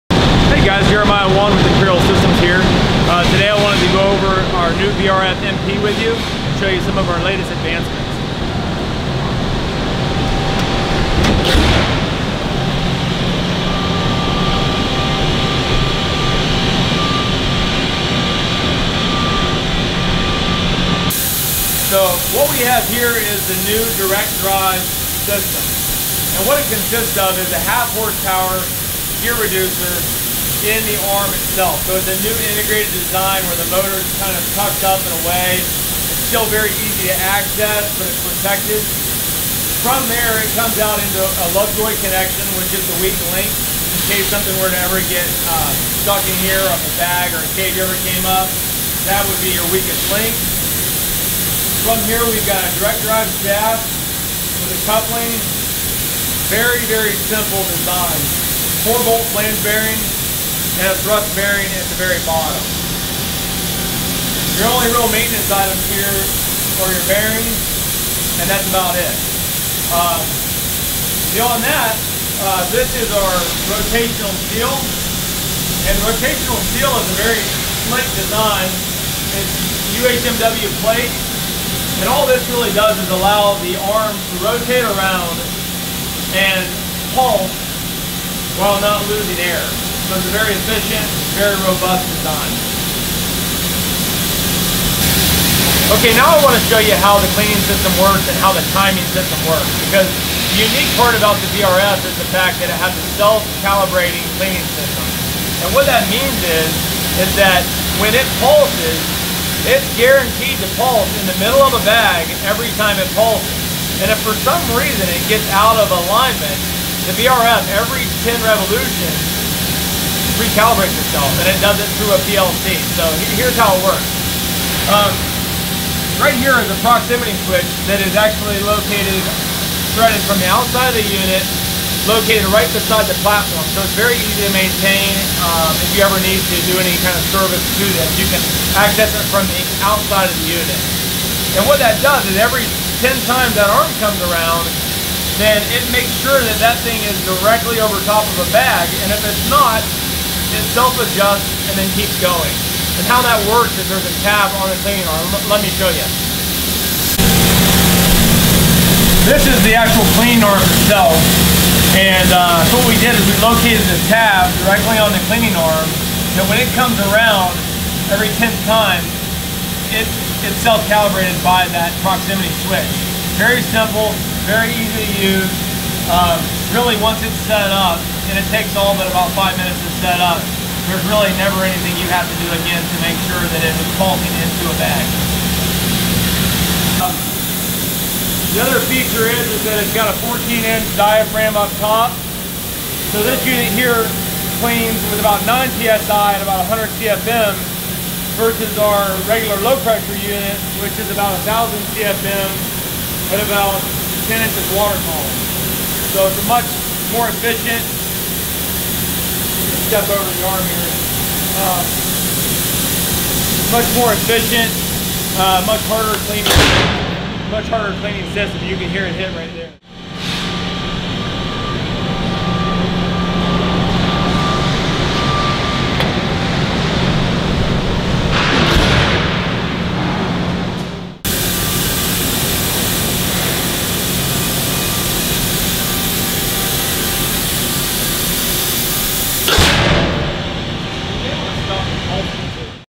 Medium Pressure Baghouse - New Features
Imperial Systems, Inc presents the redesigned Medium Pressure BRF Baghouse. The BRF baghouse is perfect for many dust collection applications. A baghouse is a dust collector that uses cloth bags as filters. With the Medium Pressure BRF, all the bag filters are cleaned with compressed air pulses. Watch the video to see how this new baghouse design improves dust collection.